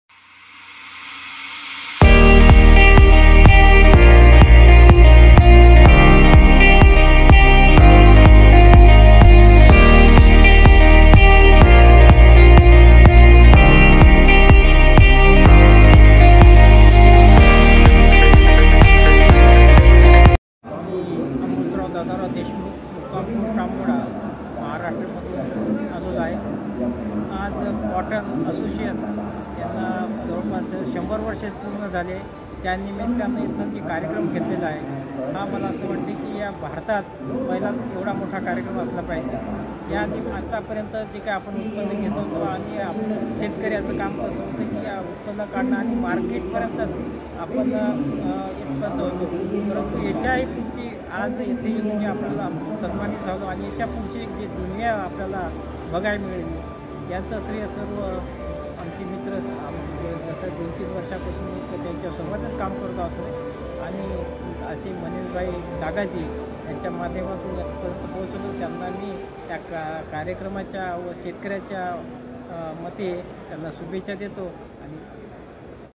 कॉटन एसोसिएशन ऑफ इंडिया शताब्दी समारोह 18 अक्टूबर 2022
सर्वश्रेष्ठ किसान पुरस्कार - प्रति एकद उच्चतम उपज
उच्चतम औसत उपजा - अमृतराव देशमुख ग्राम अम्बोदा, तालुका महागाओ, विदर्भ, महाराष्ट्र